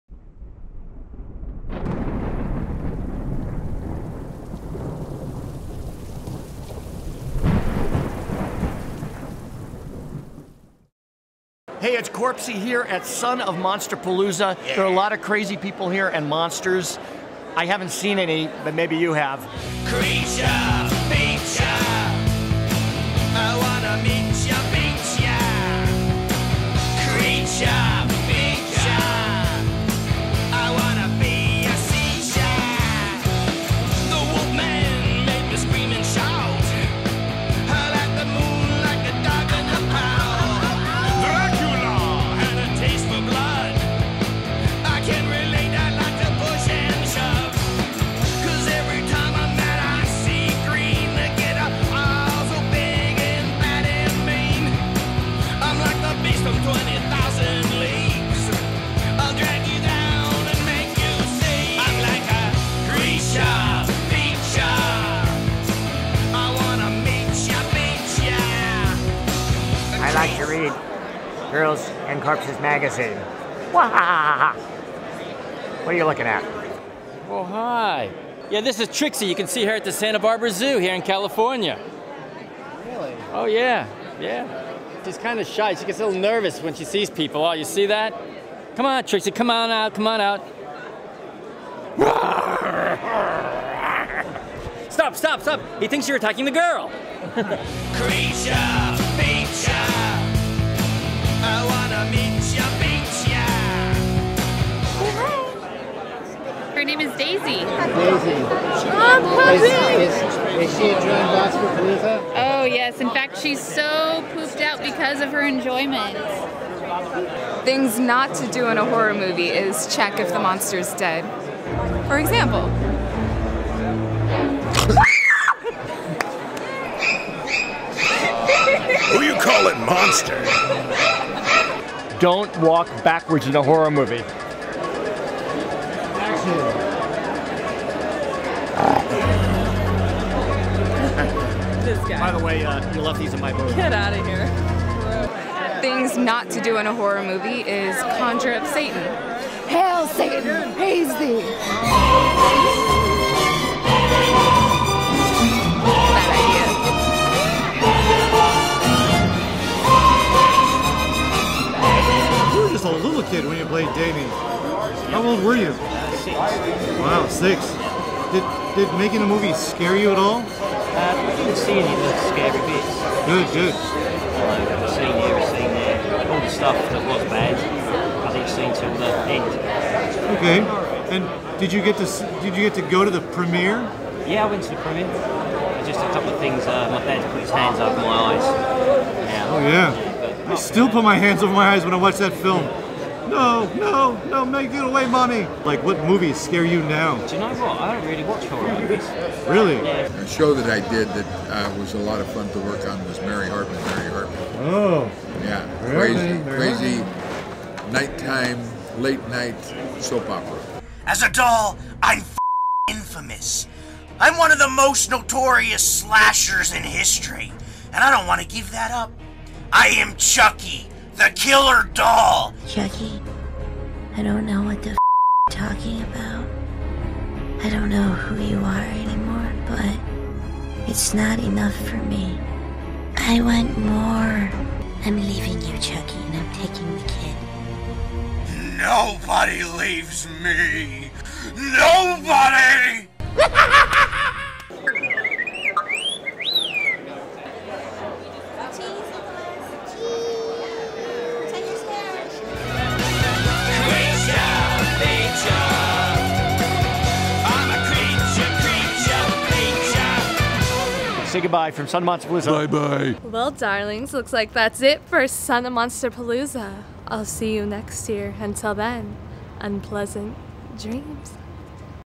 GIRLS AND CORPSES AT SON OF MONSTERPALOOZA WITH KILLER PUPPETS
Killer Puppets at Son of Monsterpalooza with Girls and Corpses Magazine.